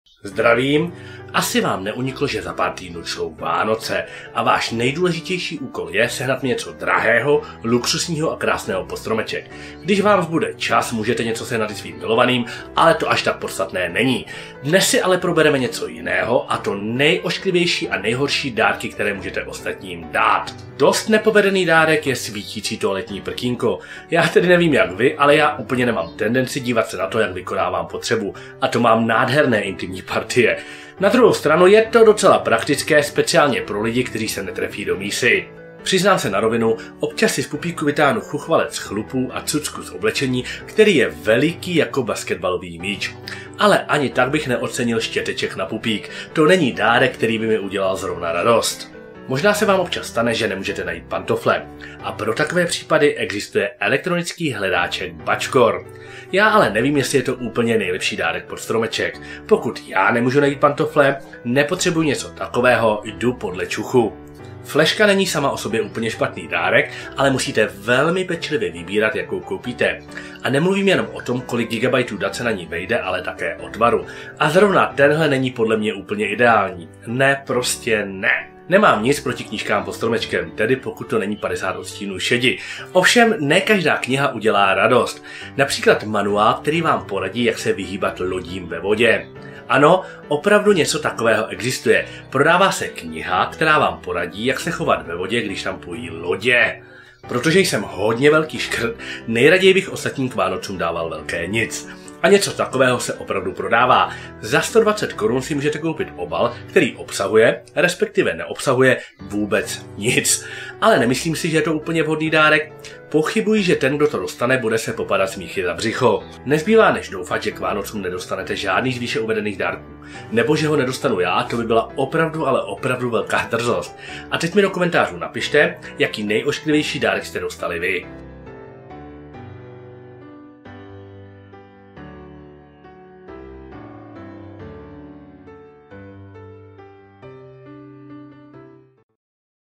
Nejhorší vánoční dárky 2
Je před Vánoci, takže se po dlouhé době vrátím k tématu, jaké jsou ty nejhorší dárky, které můžete dát nebo dostat pod stromeček :).